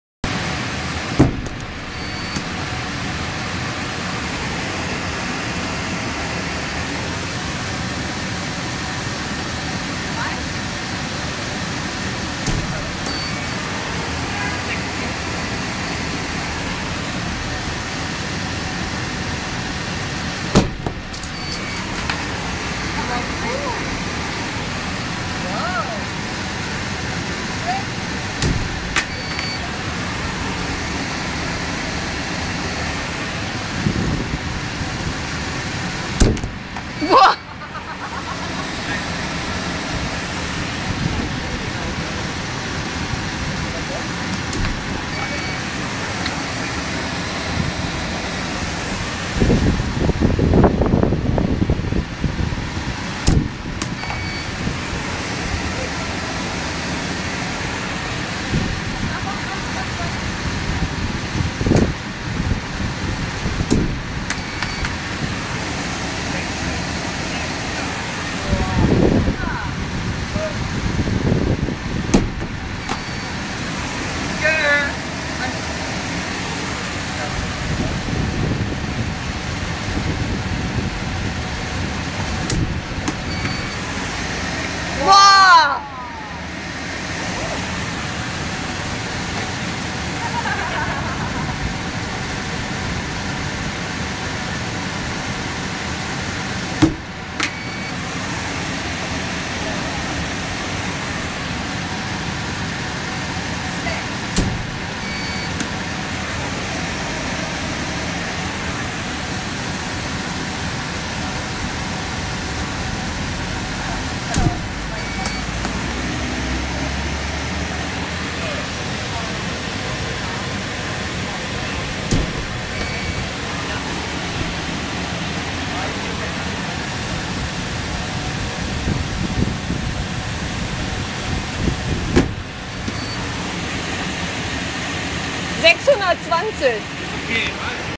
Wie schnell fliegt eine Dogfrisbee-Scheibe?
Getest auf der Radio-Tour